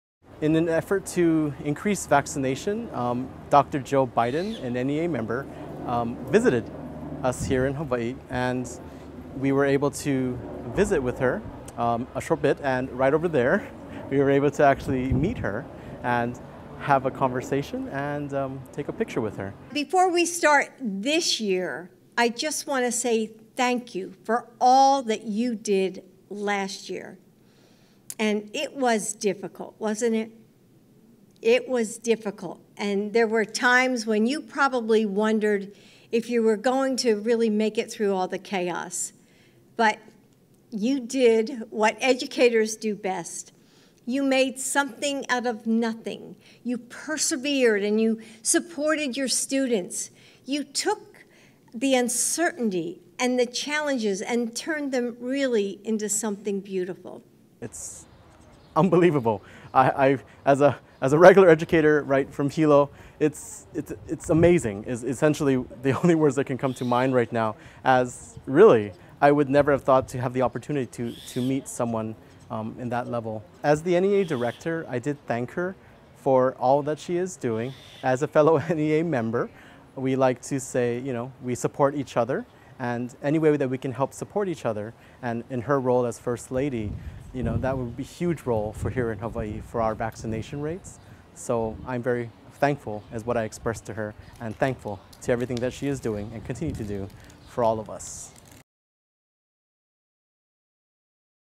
HSTA teacher leaders meet first lady Jill Biden during Hawaii visit
Several Hawaii State Teachers Association leaders met first lady Dr. Jill Biden as she toured a pop-up COVID-19 vaccination clinic at Waipahu High School Sunday.

During remarks before an audience that also included state and city lawmakers and Hawaii State Department of Education officials, Biden thanked her fellow educators for their hard work during the pandemic.

“It was difficult, and there were times when you probably wondered if you were going to really make it through all the chaos,” Biden said. “But you did what educators do best. You made something out of nothing. You persevered and you supported your students. You took the uncertainty and the challenges and turned them really into something beautiful.”

“It’s unbelievable,” said HSTA NEA Director Aaron Kubo. “As a regular educator right from Hilo, ‘It’s amazing’ is essentially the only words that can come to mind right now as really I would never have thought to have the opportunity to meet someone in that level.”

Biden, a community college professor and longtime member of the National Education Association, made a brief stopover on Oahu following a visit to Tokyo for the 2020 Olympics. Her visit to Waipahu High School is part of the administration’s nationwide effort to reach millions of Americans who still need protection against the virus and highlight the ease of getting vaccinated.